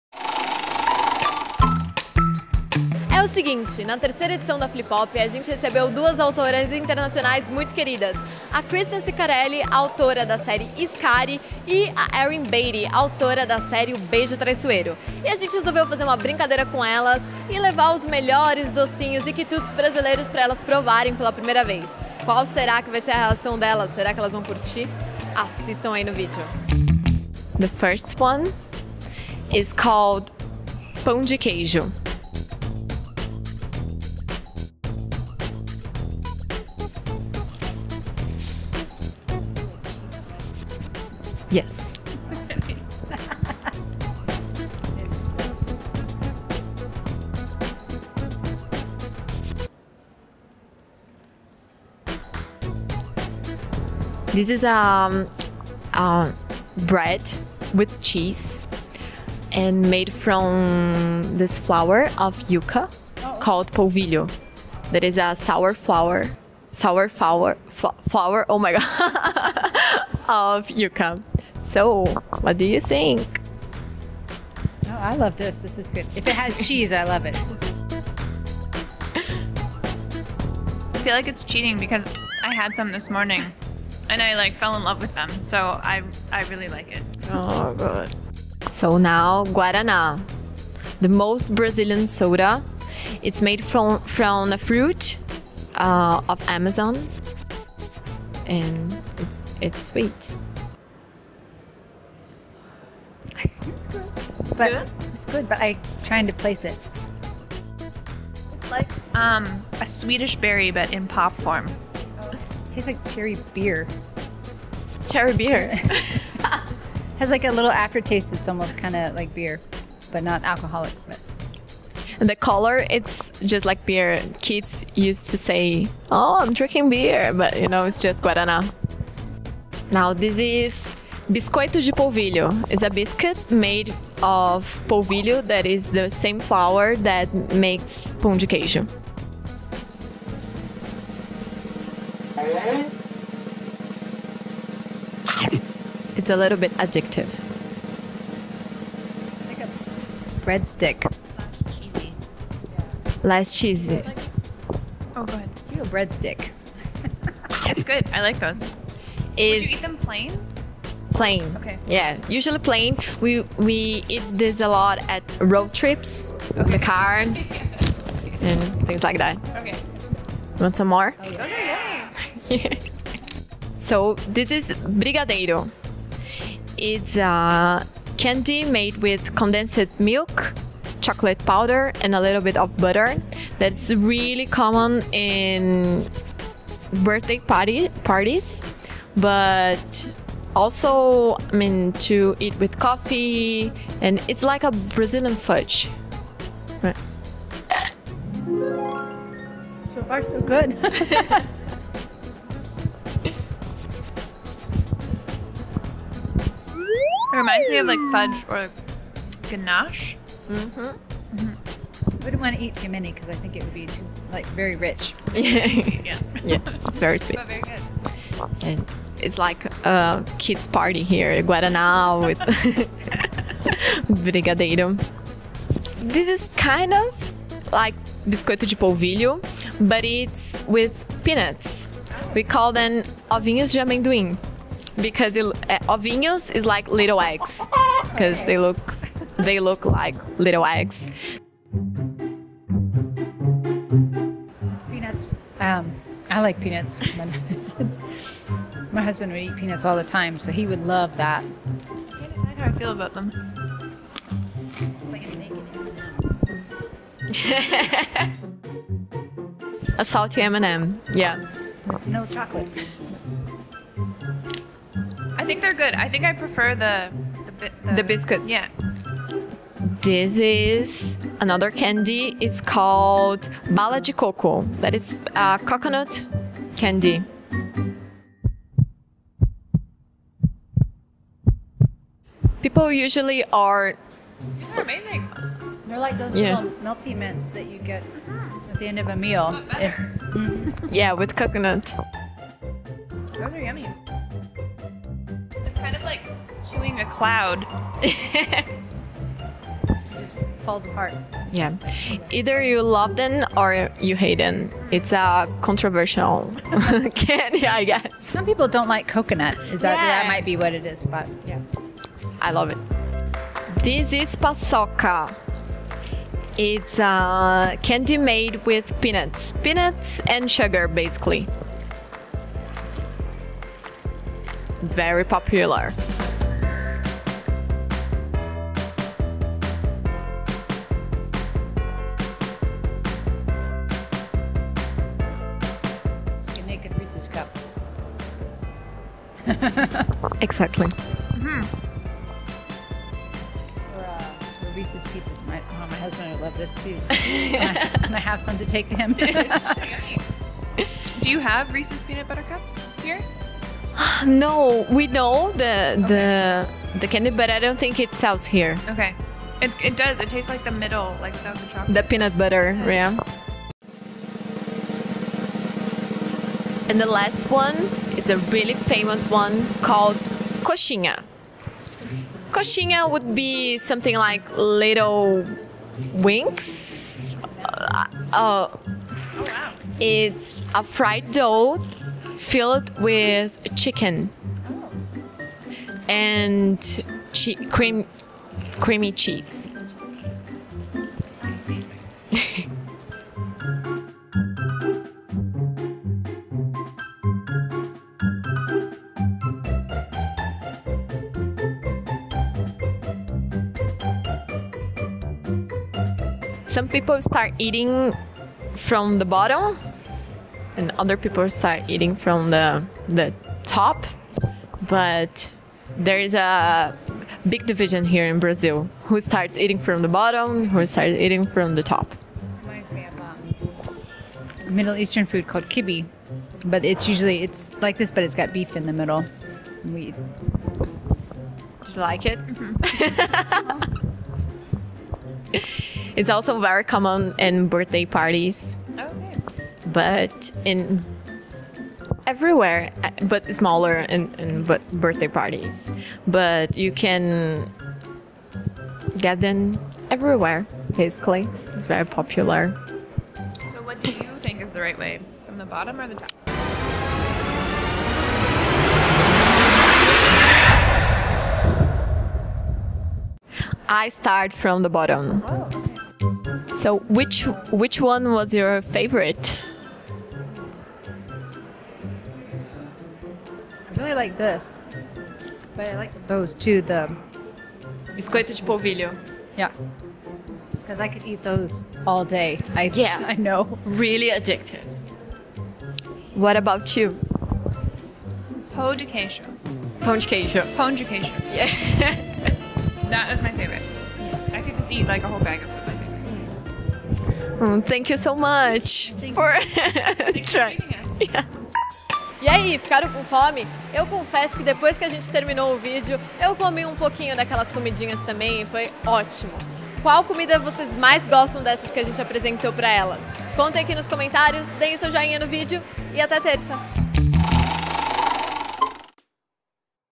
Experimentando comidas brasileiras: Erin Beaty e Kristen Ciccarelli | FLIPOP 2019
ATIVE AS LEGENDAS PARA ACOMPANHAR EM PORTUGUÊS

As autoras de "O beijo traiçoeiro" e "A caçadora de dragões" vieram ao Brasil para a FLIPOP e aproveitaram para conhecer algumas guloseimas do nosso país.

Não deixe de acompanhar a Seguinte nas outras redes!